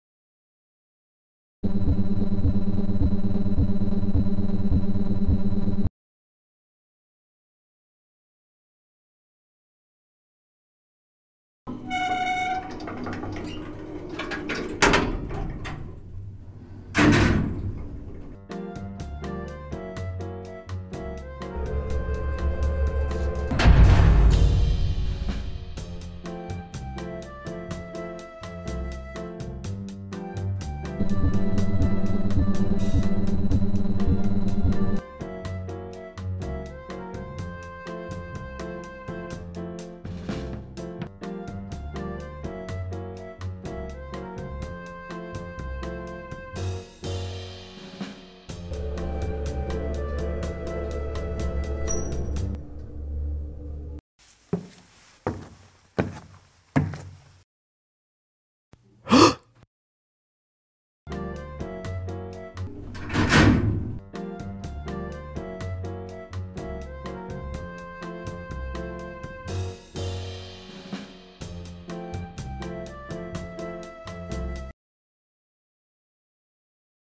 Liftā
TJN "Annas 2" animācijas pulciņa audzēkņu darbi, 2023. gads.